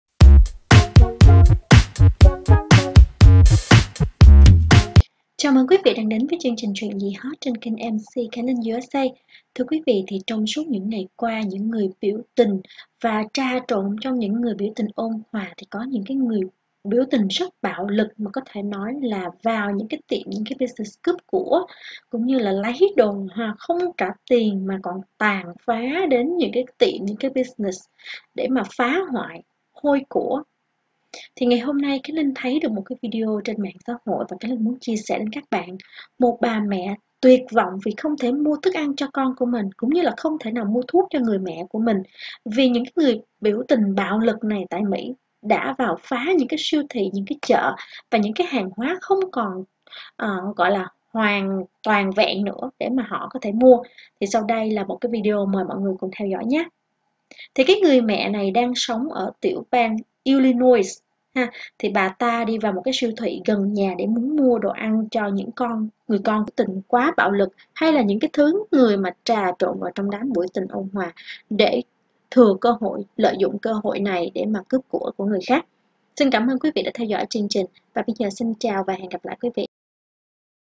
CÁC CHỢ BUSINESS BỊ ĐẬP PHÁ VÌ BIỂU TÌNH BẠO LỰC TẠI MỸ ll Chuyện Gì HOT?
***Thưa các bạn và các fan yêu quý, trong suốt những ngày qua những cuộc biểu tình và bạo loạn được xảy ra khắp nước Mỹ, kênh MC Khánh Linh đã gửi đến các bạn những tin tức cũng như hình ảnh tàn phá đau thương cập nhất để mọi ngừơi biết và cẩn thận. Nhưng chắc đến thời điểm này ai cũng đã hiểu rõ và ko muốn nhắc lại để bị ám ảnh về điều này cũng như tạo cơ hội cho các phần tử cực đoan chiếm lấy social media...nên MC Khánh Linh sẽ xoá những hình ảnh đau buồn này nhé ! Cám ơn mọi người ủng hộ ý kiến này của MC Khánh Linh.

Xem xong cảnh này mọi người hãy đồng cảm với bà mẹ này vì nếu sáng mai ra chợ mua sữa cho con, mua thuốc cho mẹ già đang bệnh mà có tiền trong tay vẫn ko mua được gì. Bạn sẽ phải làm sao? Hãy lên án những kẻ tàn phá ngông cuồng này...cầu mong 1 nước Mỹ yên bình trở lại...để không thôi TQ lại dậy sóng...
Hãy chia sẻ cho bạn bè cùng xem. Thanks very much!